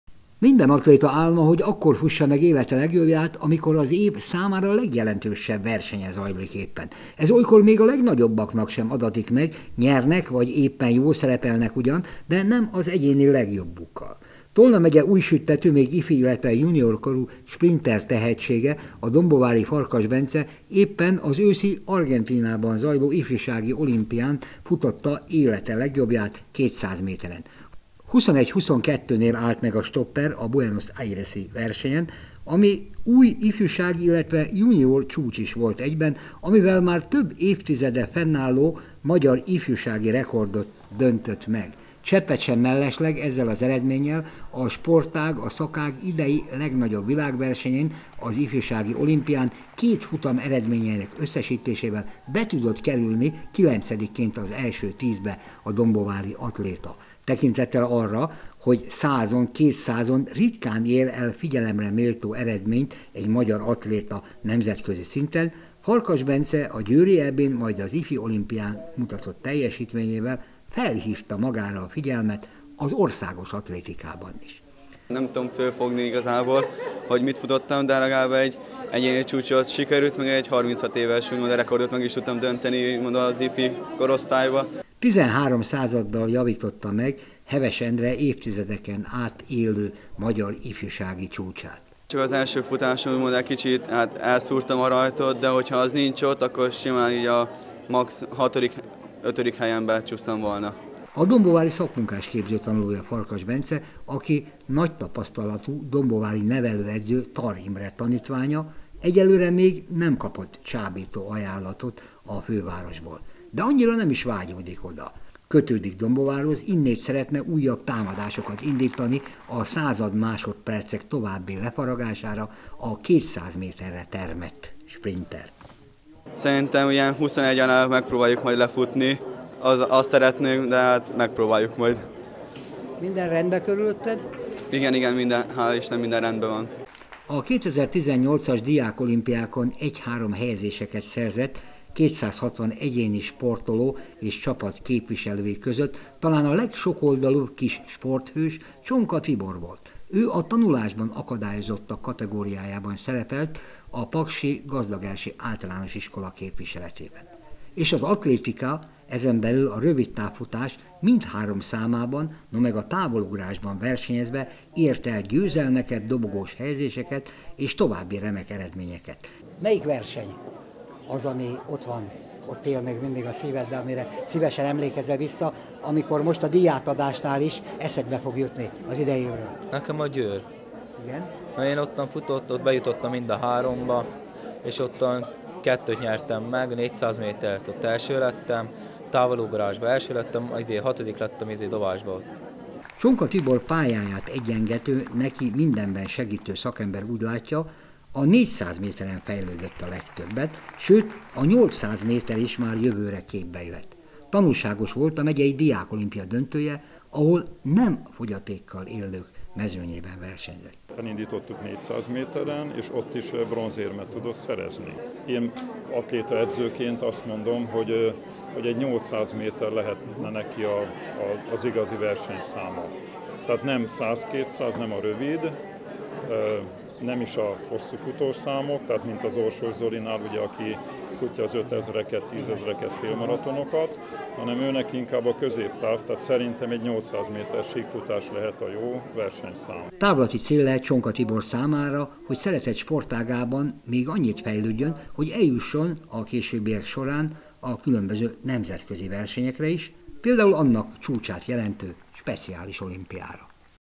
Tolna Megyei Diáksport Tanács a szokásos év végi ünnepség. 20181225
A hazai diákolimpiákon dobogós helyezést elért több, mint kétszázötven sportolót köszöntött és díjazott a Tolna Megyei Diáksport Tanács a szokásos év végi ünnepségén. Közülük ezúttal kettővel ismerkedhetnek meg egy kicsit bővebben. Az egyik a tavalyi év legnagyobb nemezetközi versenyén, az argentinai ifjúsági olimpián is remekelt, a másik egy olyan sokoldalú, fogyatékkal élő, egész pontosan tanulásban akadályozott sportoló, aki az atlétikában már olyan jó eredményeket produkál, ami az ép versenyzőknek is dícséretére válna.